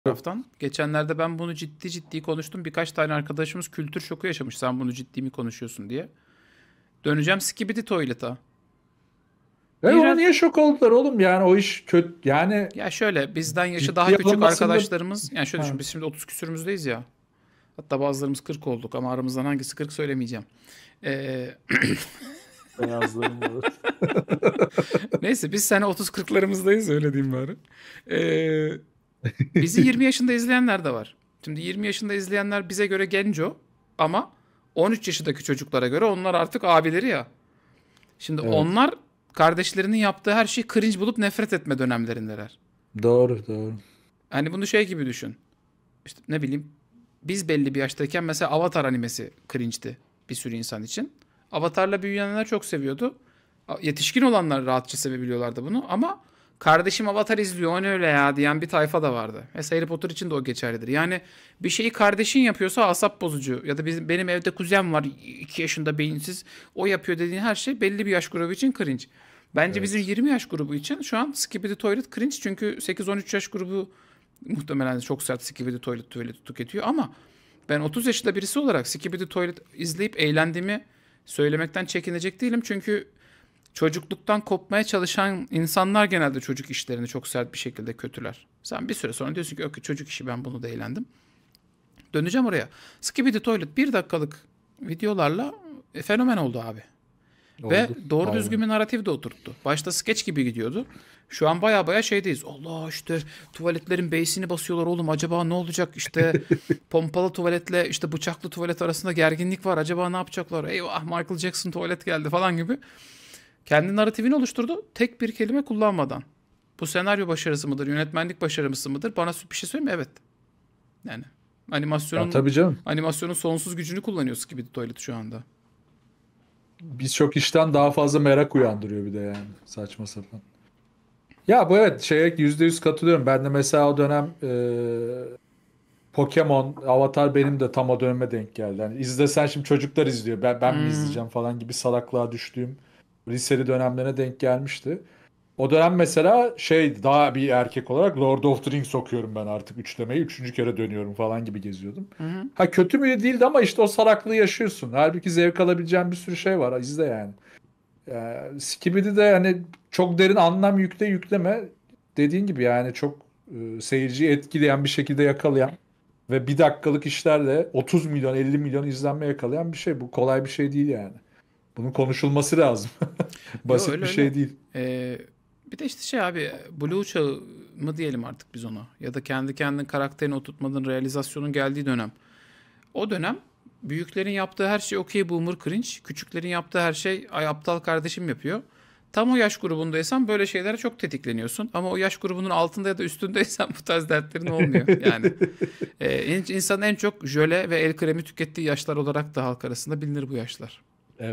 Can Sungur "Skibidi Toilet" sevmeyenlerin maskesini nasıl düşürdü?
Skibidi Toilet yüksek oktanlı, gayet boktan bir şeydir...